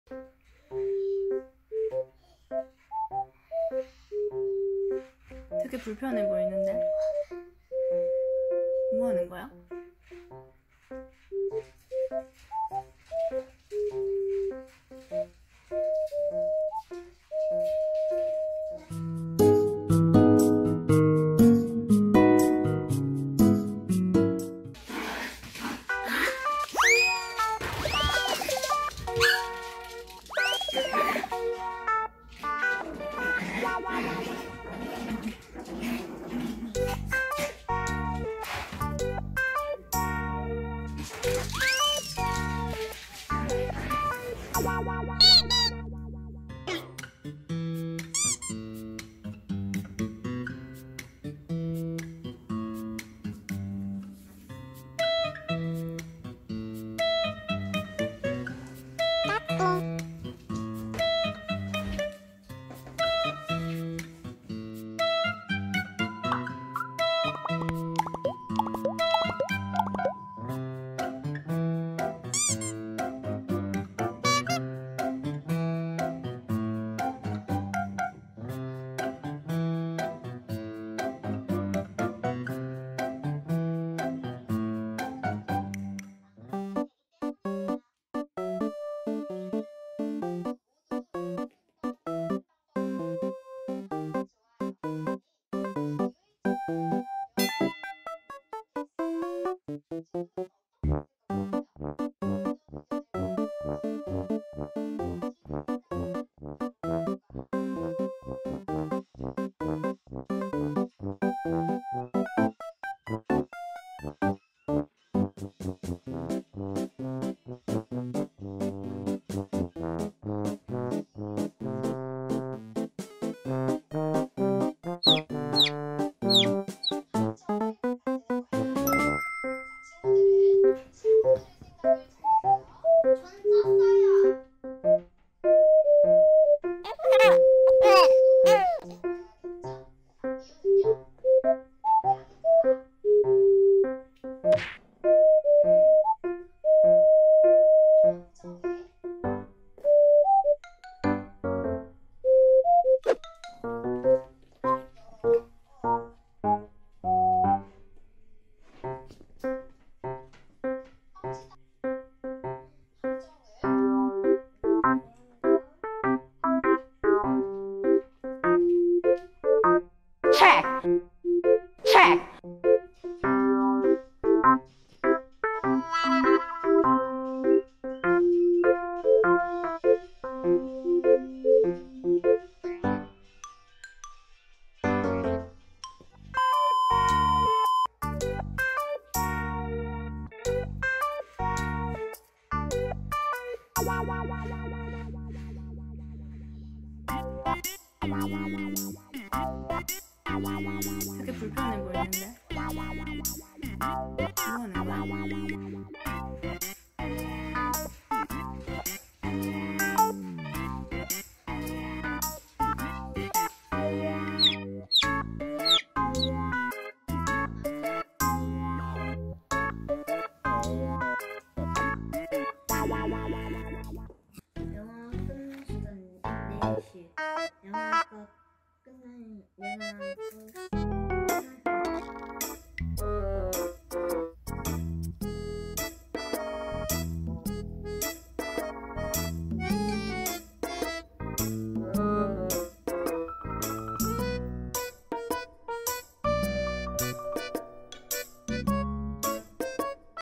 포메라니안 가정다견의 소소한 일상
오늘은 포메라니안 가정다견 모찌레나와 조이의 소소한일상이에요~

다음에는 더 재미있는 영상으로 찾아올게요~